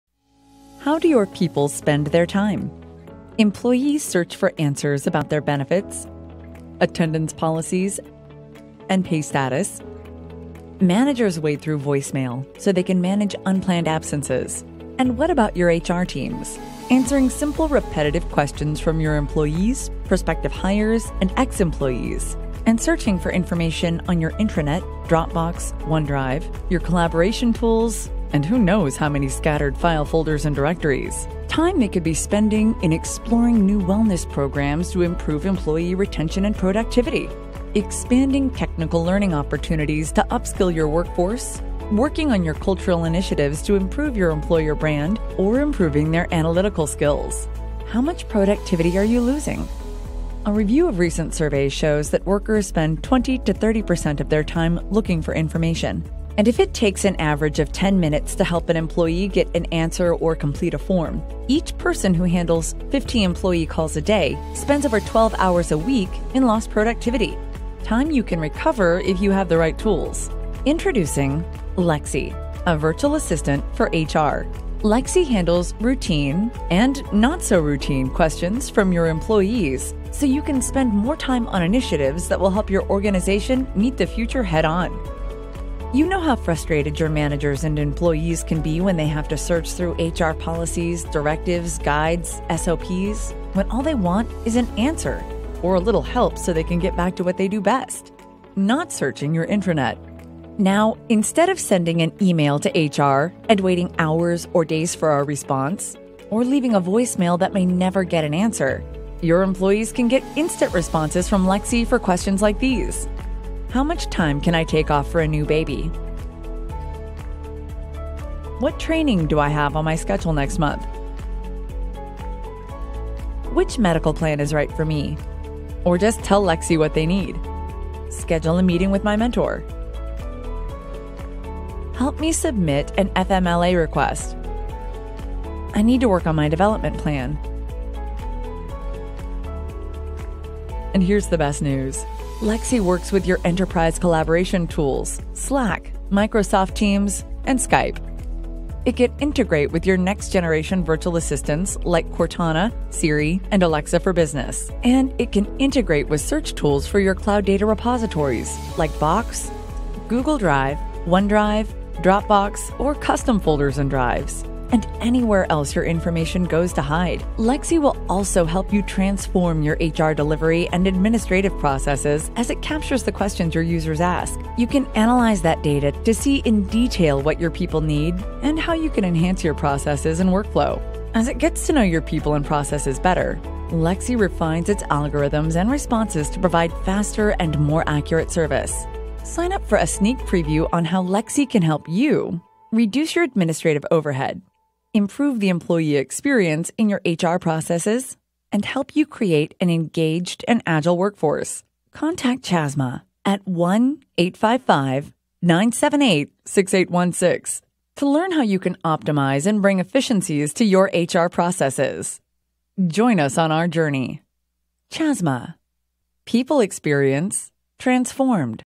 Transform HR Process delivery with Chasma Lexy
Lexy - your new HR virtual assistant can help your people get instant answers instead of waiting for an email or callback from support staff, request any resource instead of wading through lists, and access learning, benefits enrollment, attendance, performance management, or any other task in your human capital management systems.